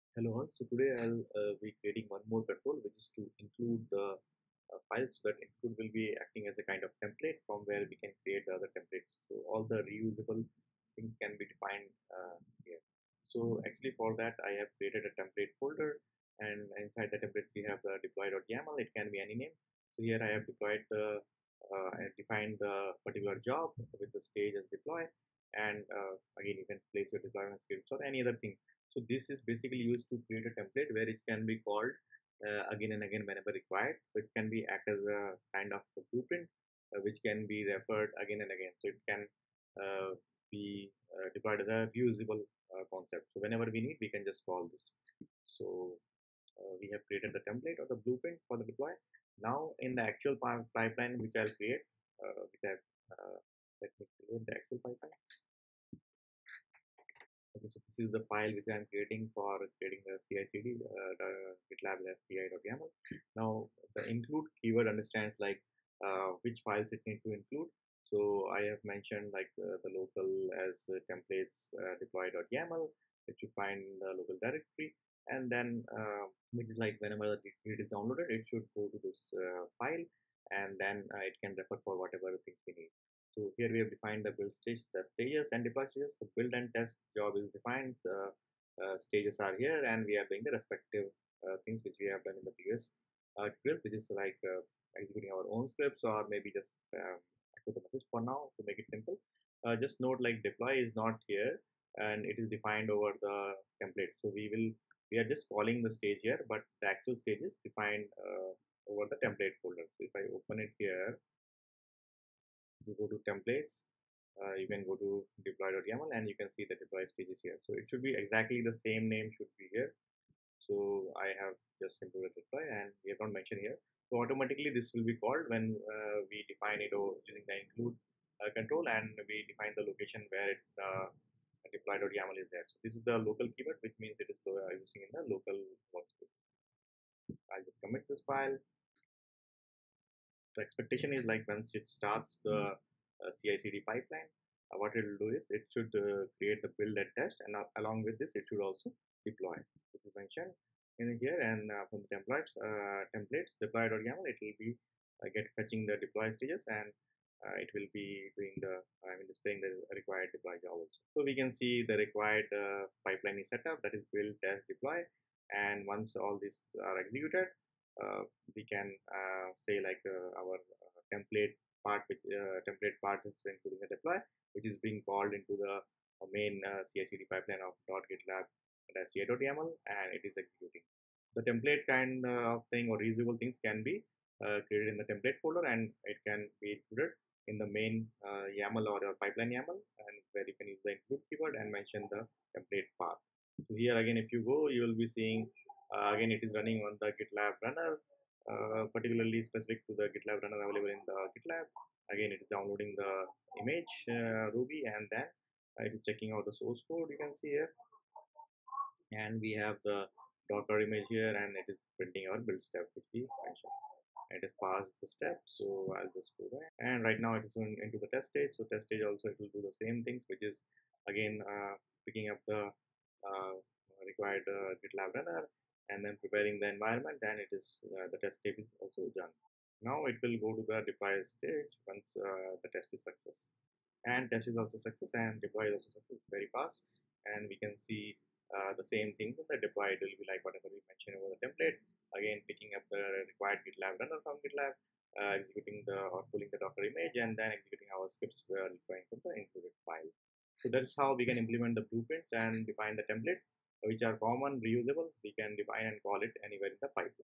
GitLab CI Pipeline Templates with Include | Reusable CI/CD Blueprints in GitLab
Learn how to create reusable GitLab CI/CD pipeline templates using the powerful include: feature. In this video, I walk you through a real-world implementation of GitLab pipeline blueprints that improve efficiency, scalability, and maintainability for your DevOps workflows.

You'll learn:

What are GitLab pipeline templates and blueprints

How to structure .gitlab-ci.yml files using include:

Types of include: (local, remote, template, project)

Best practices for reusable DevOps CI/CD pipelines

Real-world examples and folder structures

💡 Ideal for DevOps Engineers, SREs, and teams managing multiple projects in GitLab.

📌 Watch till the end for bonus tips on organizing pipelines across microservices and monorepos!